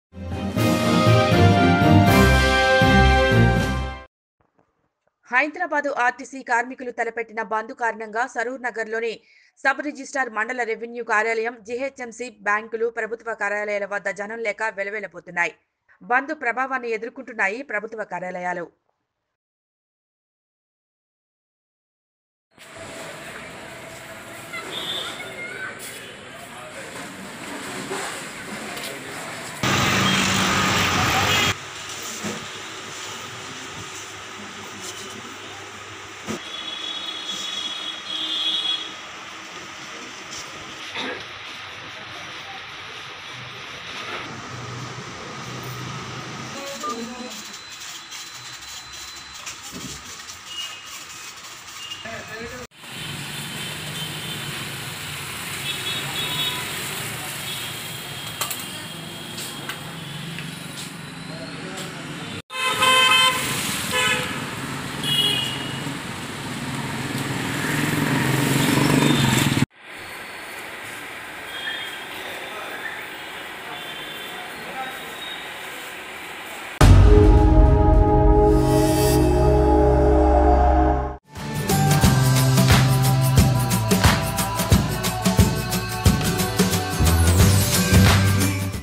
బందు కారణంగా సరూర్ నగర్ లో జనం లేక వేల వేలబోతున్న ప్రభుత్వ కార్యాలయాలు || Nethra News
|| Nethra News || Saroor Nagar||